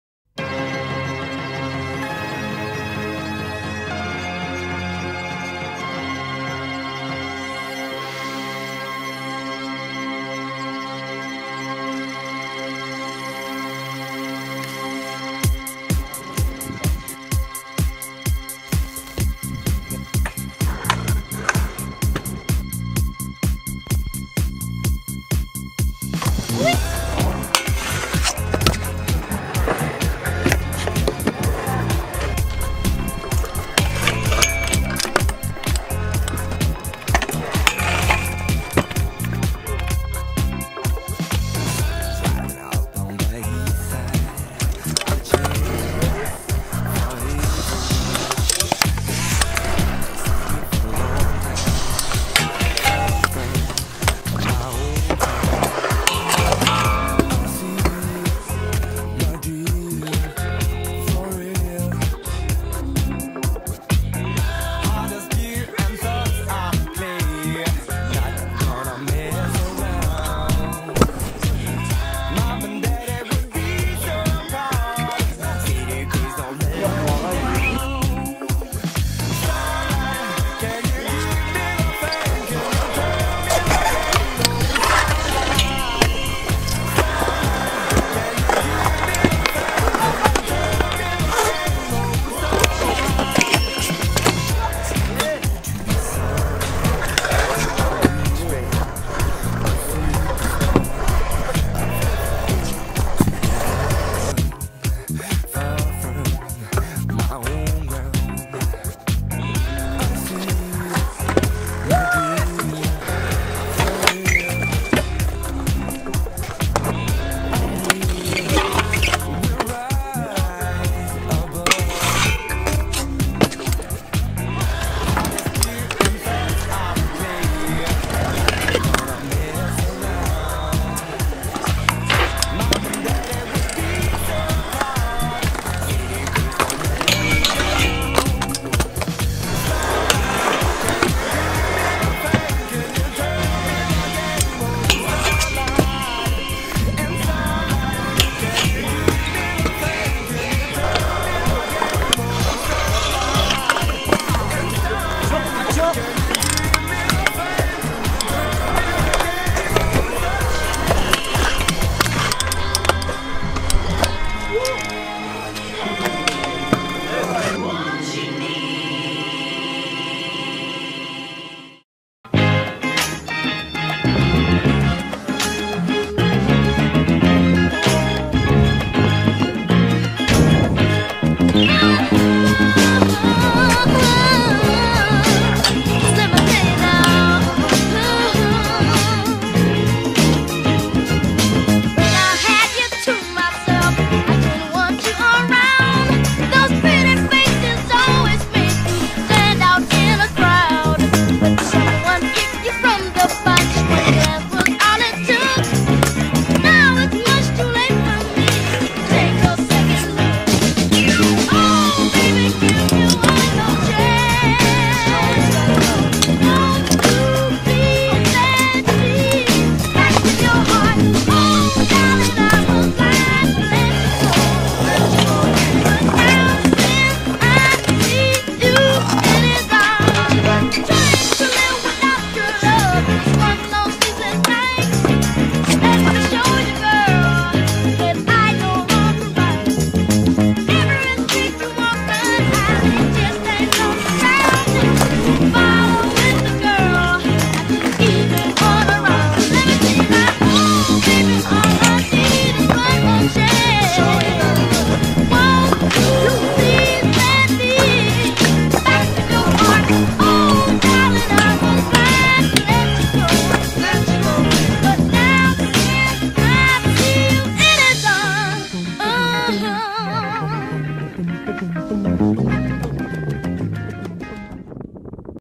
Arcachon X Bdx | Holiday Trip
Vidéo récapitulative de nos vacances en Gironde durant le mois d’août.